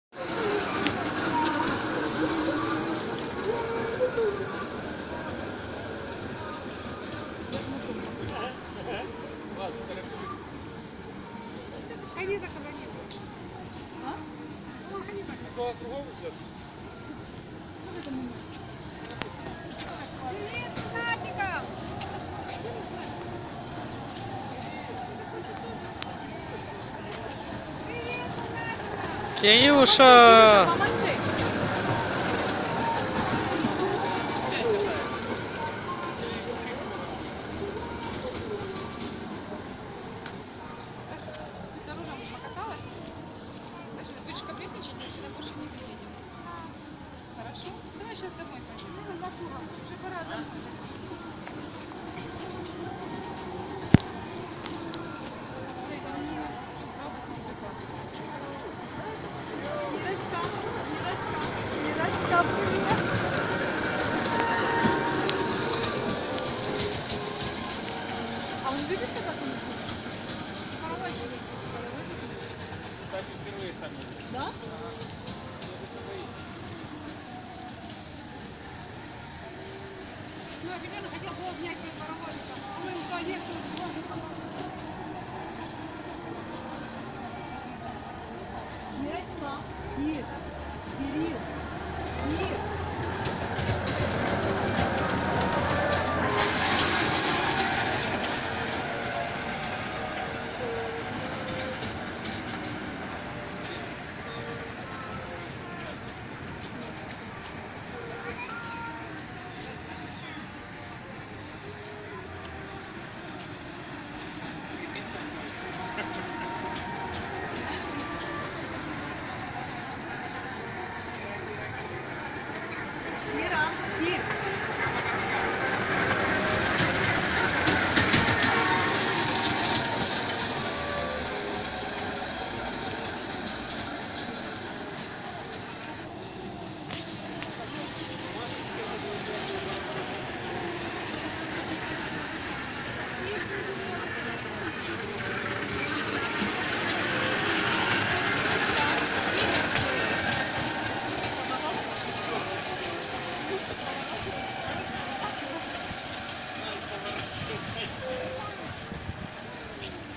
Маленький машинист. Детский развлекательный парк Сказка. г. Николаев
Кирюша первый раз самостоятельно катается в парке развлечений на поезде.